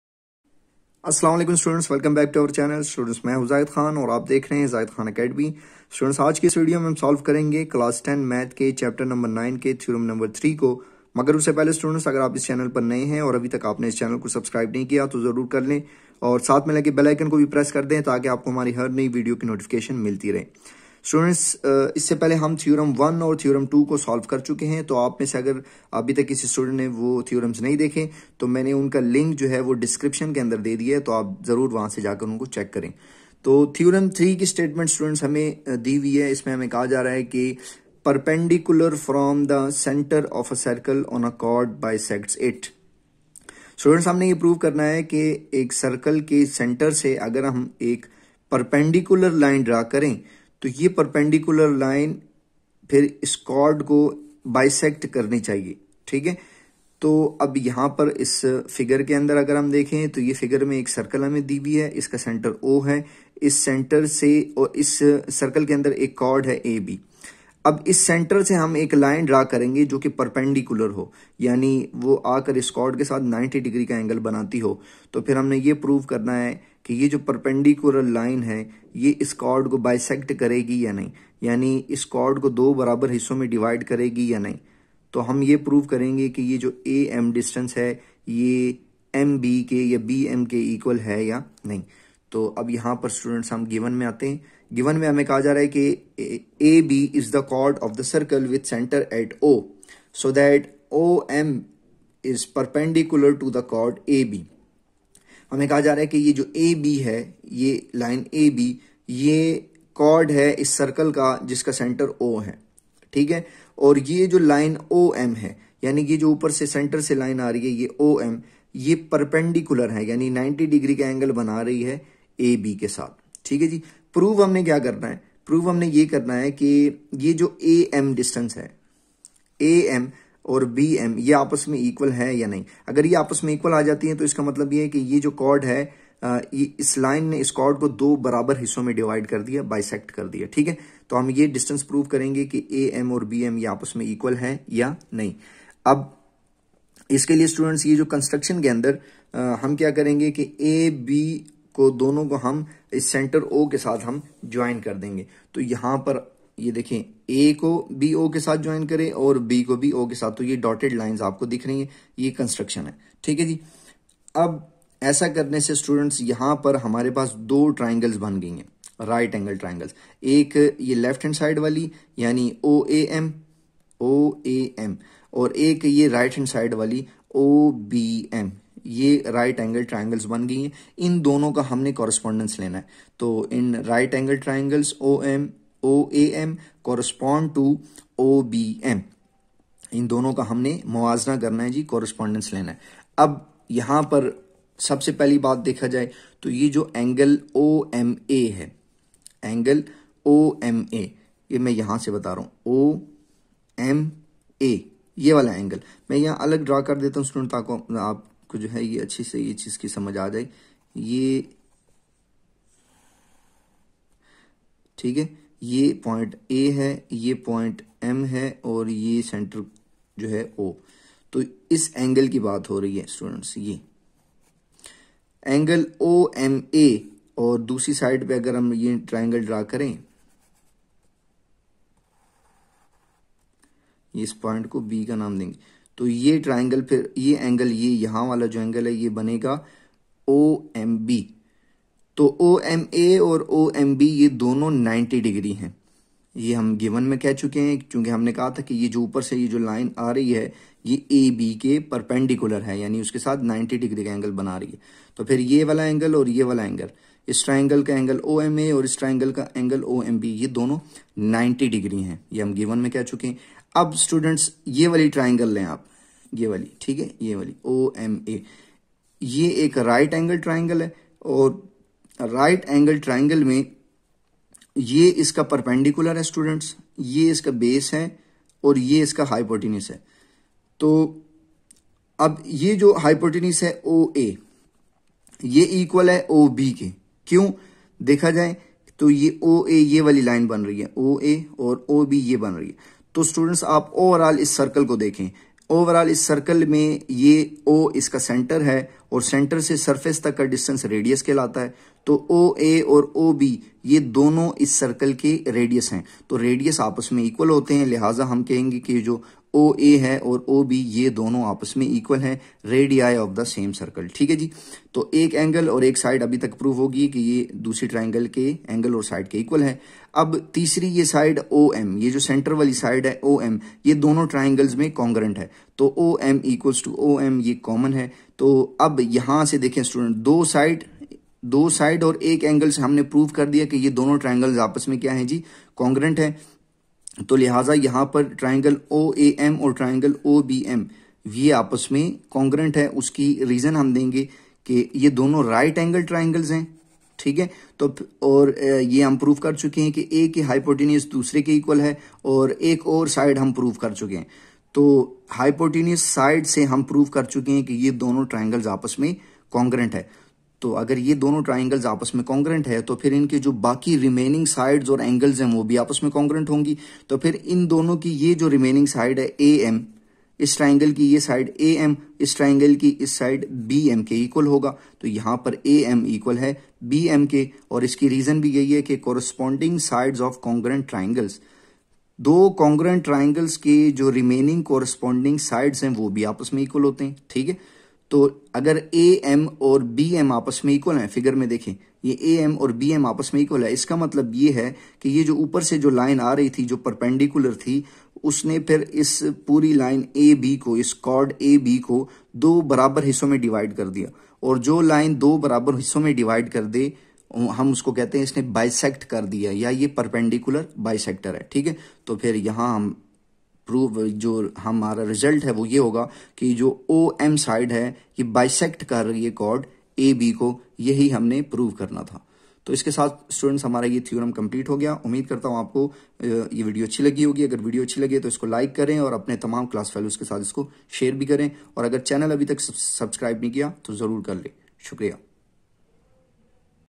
Class 10 Math Chapter 9 Theorem 3 | 10th Math Chapter 9 Theorem 3 | Zahid Khan Academy
Hi Students !! In this video I've explained in detail Class 10 Math Chapter 9 Theorem no 3.